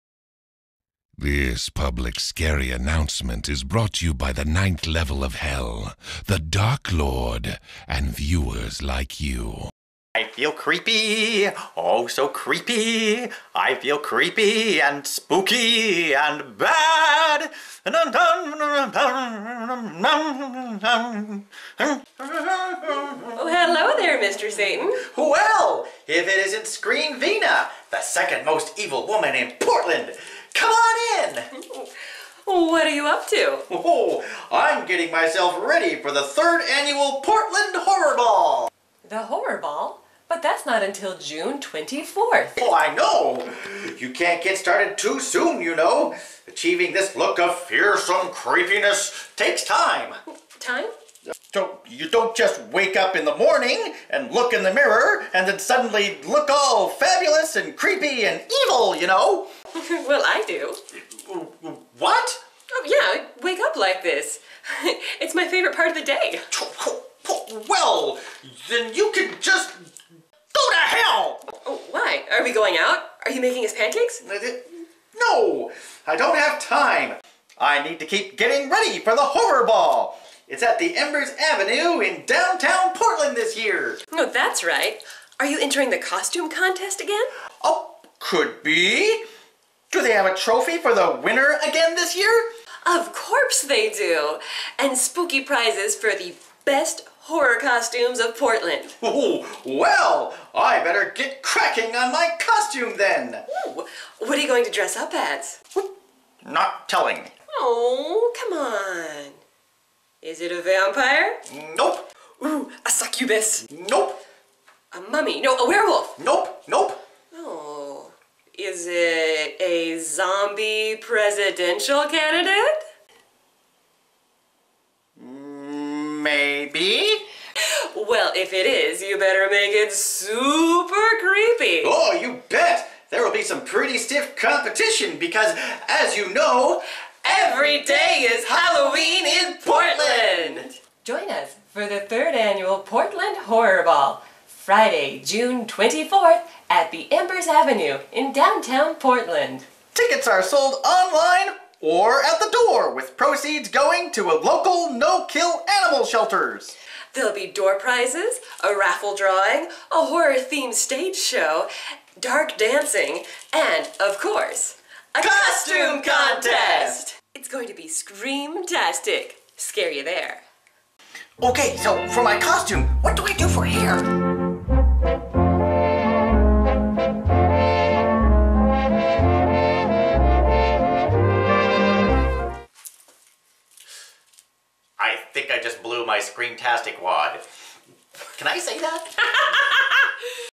3rd Annual Portland Horror Ball Promo Video
3rd Annual Portland Horror Ball at the Embers Avenue.
Friday June 24, 2016 in Portland Oregon
Hosted by Screamvina